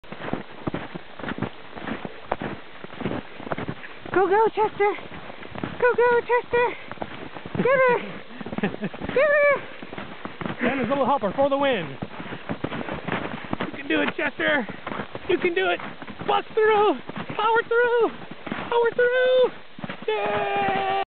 Adventureweiner Dachshunds Race Each Other in the Snow
We were out hiking in the snow with our dogs, Gretel and Chester. This is a funny video of them in a snow track racing each other. They both like to be first. They can run faster than me on snowshoes!